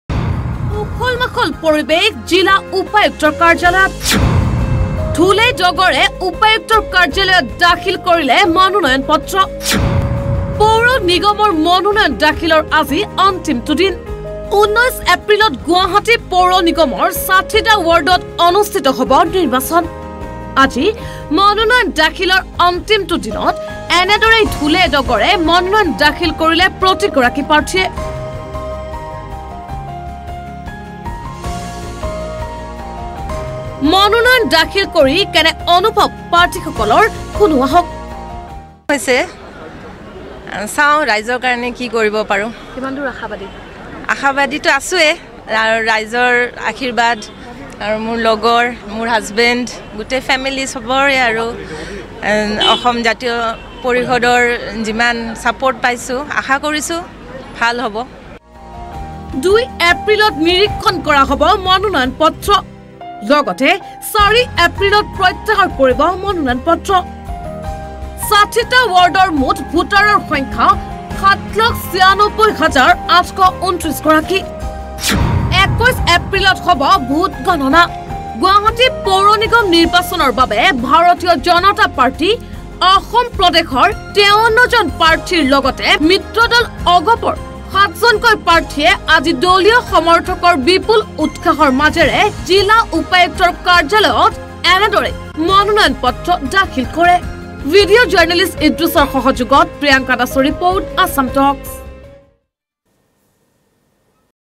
১৯ এপ্ৰিলত গুৱাহাটী পৌৰনিগমৰ ৬০টা ৱাৰ্ডত অনুষ্ঠিত হ’ব নিৰ্বাচন
১৯ এপ্ৰিলত গুৱাহাটী পৌৰনিগমৰ ৬০টা ৱাৰ্ডত অনুষ্ঠিত হ’ব নিৰ্বাচন

আজি উখল-মাখল পৰিৱেশে বিৰাজ কৰিছে জিলা উপায়ুক্তৰ কাৰ্যালয়ত